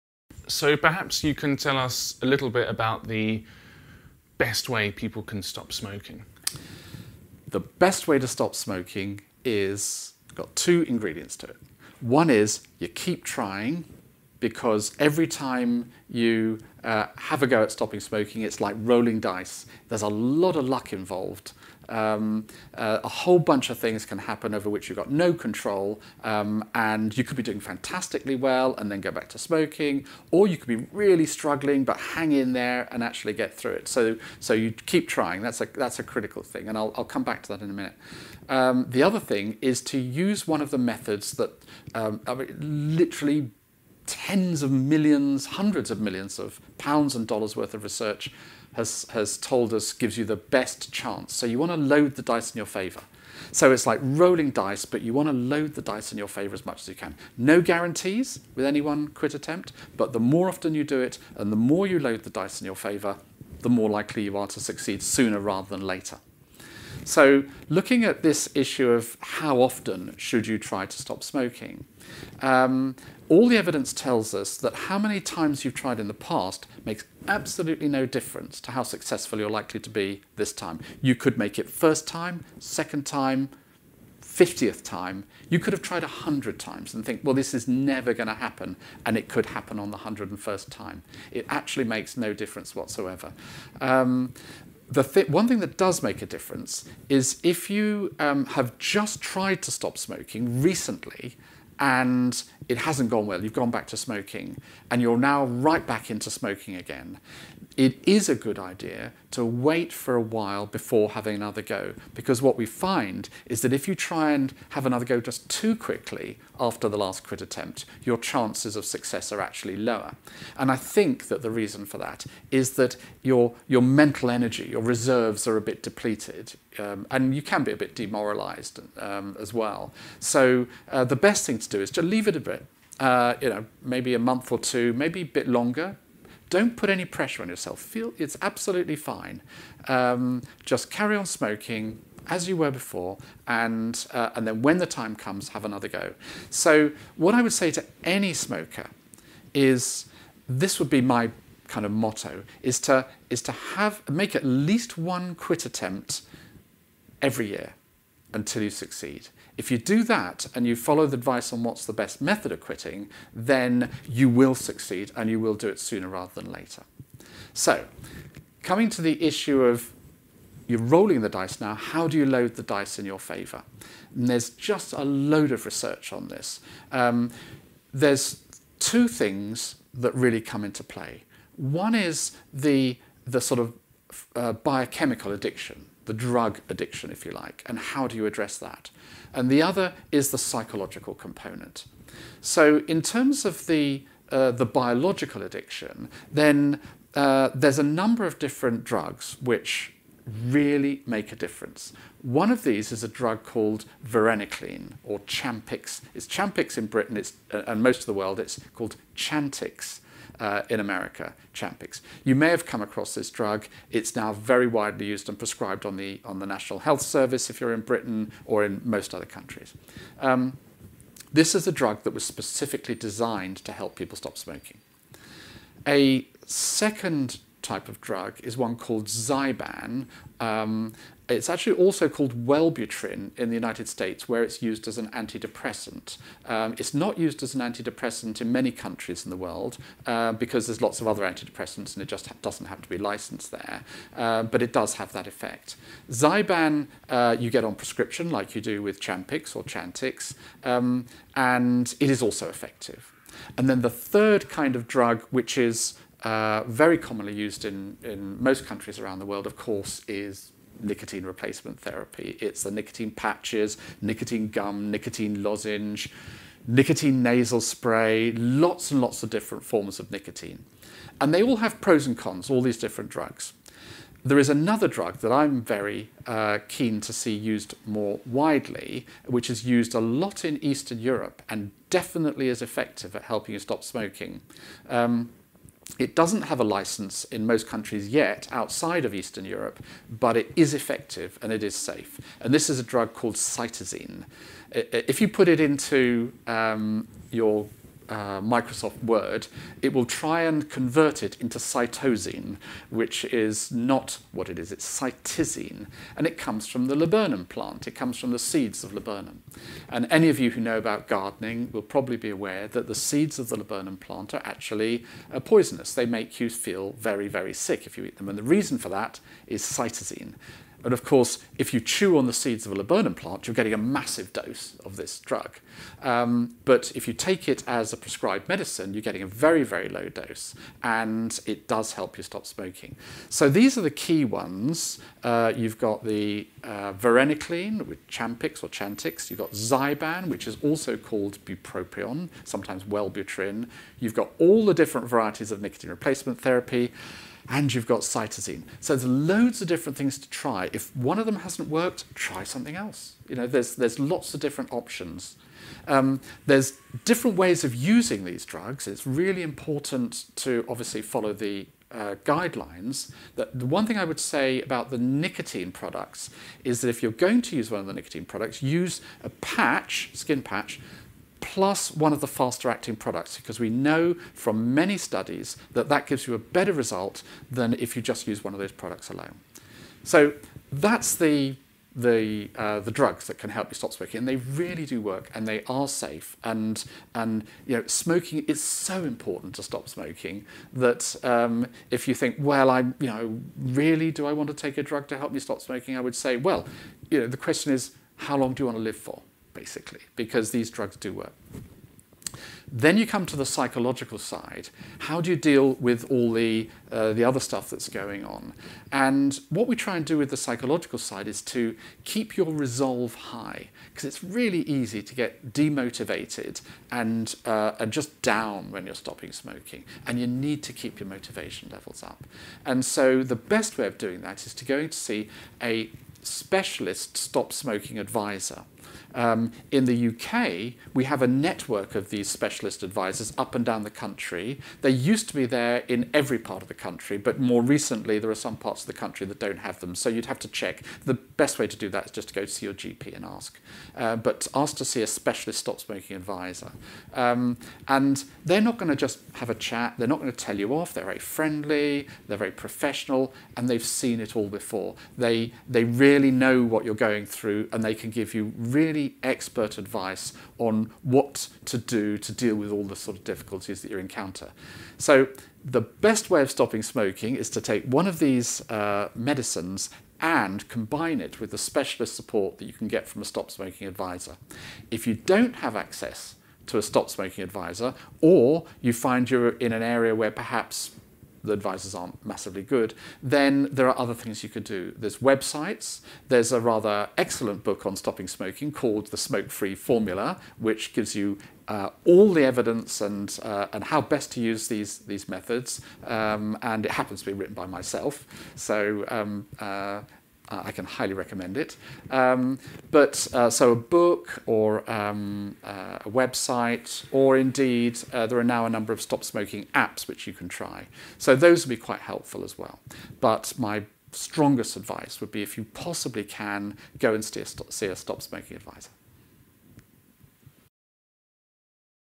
Best ways of stopping smoking
Third of a series of short videos explaining about why people smoke and what they can do to stop - according to the scientific evidence thus far. I am a Professor at University College London who has been researching the topic for more than 30 years. for more information.